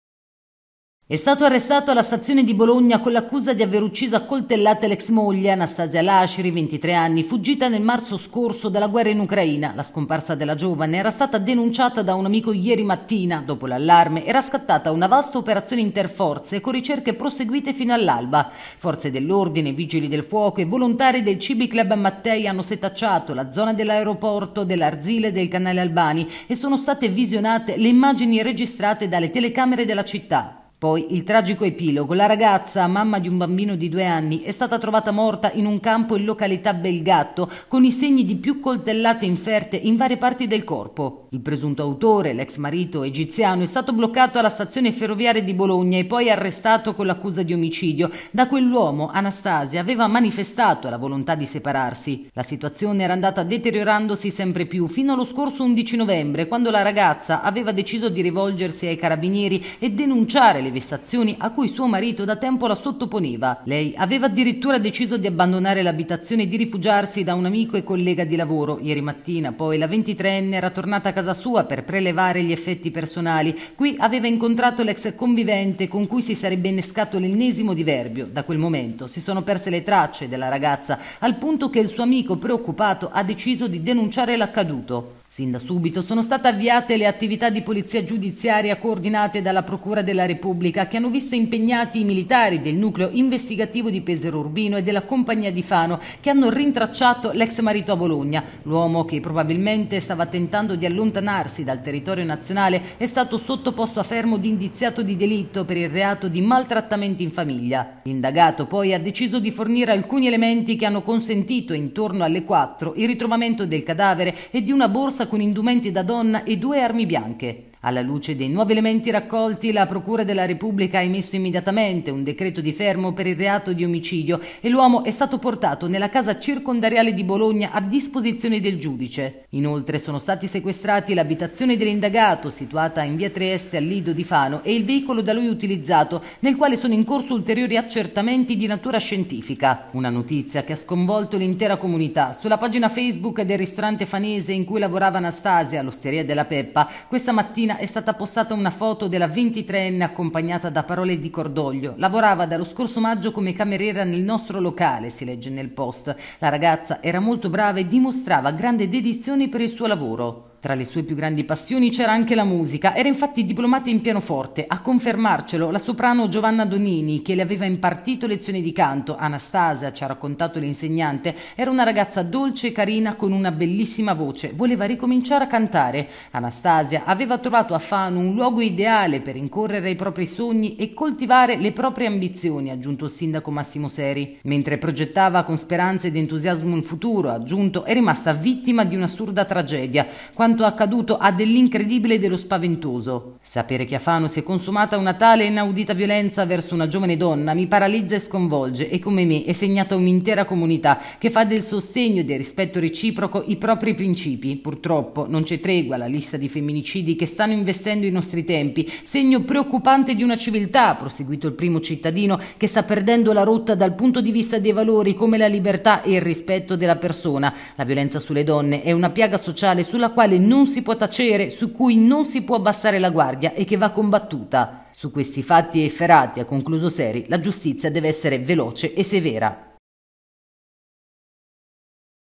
23enne uccisa a Coltellate a Fano
Trovata morta accoltellata Anastasiia Alashri, ucraina di 23 anni. Il presunto autore dell’omicidio, l'ex marito, è stato bloccato alla stazione di Bologna. L’uomo era già stato denunciato dalla donna per maltrattamenti.